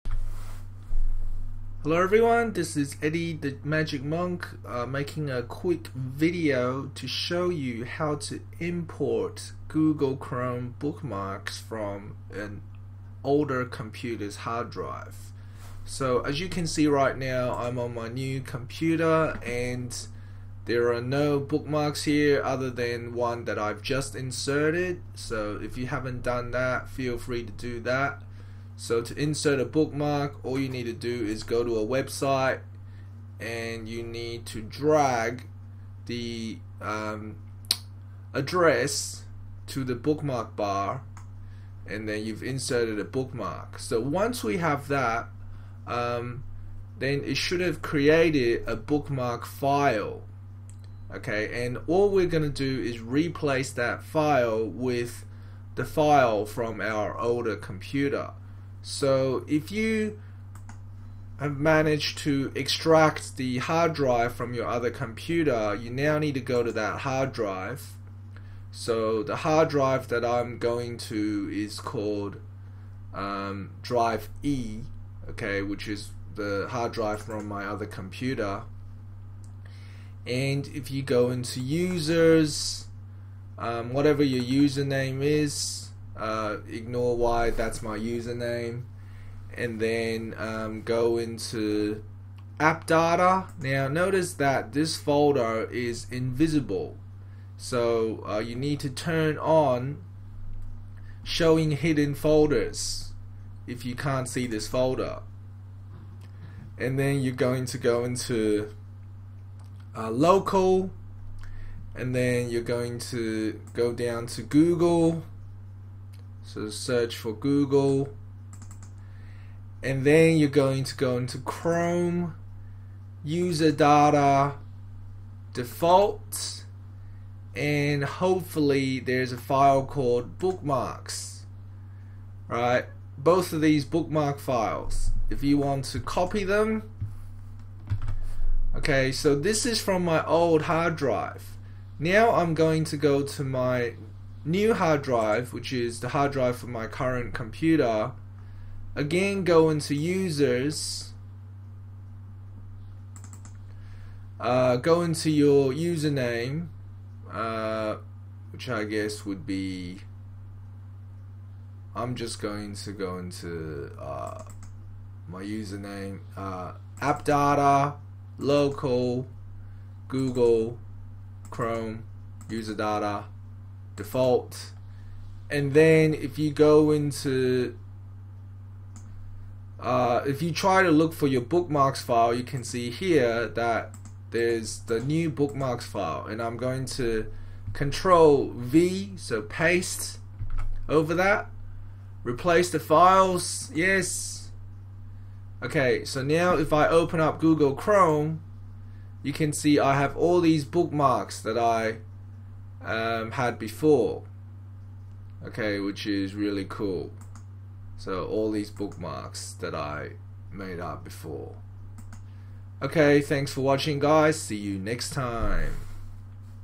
How to recover chrome bookmarks stored locally on an old hard drive (not using google account)
One of my old laptops just went bust and I had a lot of bookmarks that were saved on it in Chrome. This video will show you how to recover them.